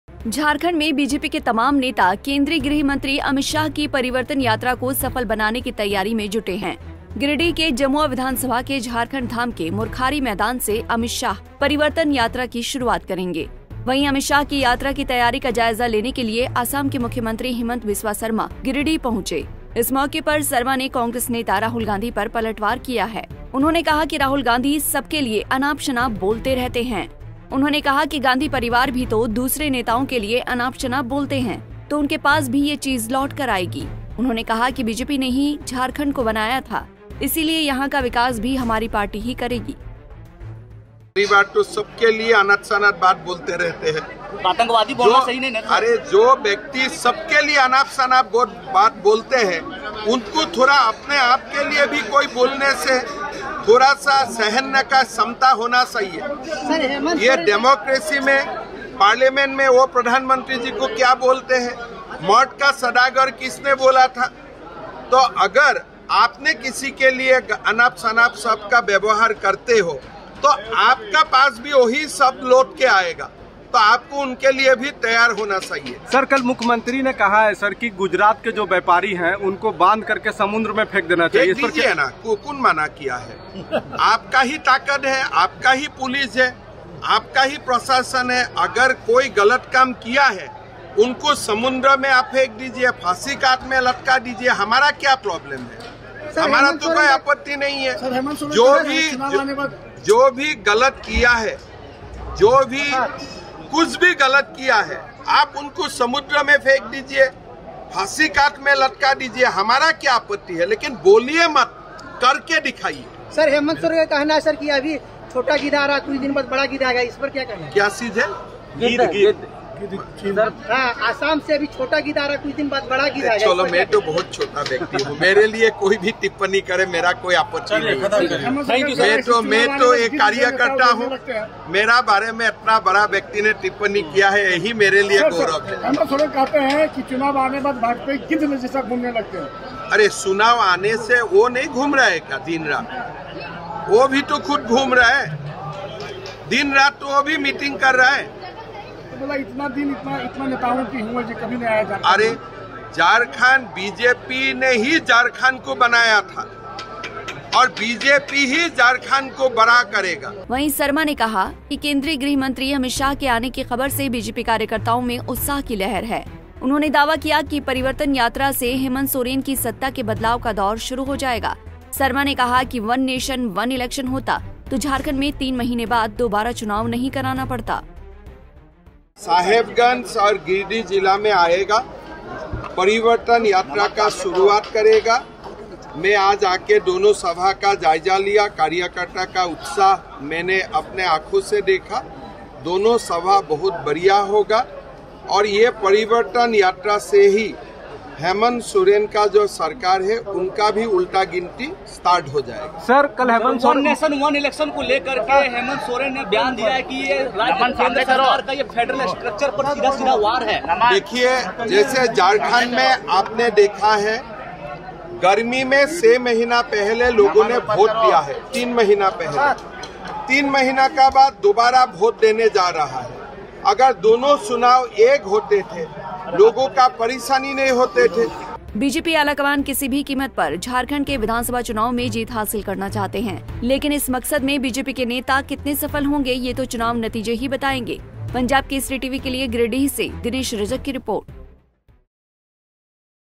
असम के मुख्यमंत्री हिमंता बिस्वा सरमा का कांग्रेस पर वार
असम के मुख्यमंत्री हिमंता बिस्वा सरमा का कांग्रेस पर वार, बोले- ‘राहुल गांधी तो सबके लिए अनाप शनाप बोलते रहते हैं’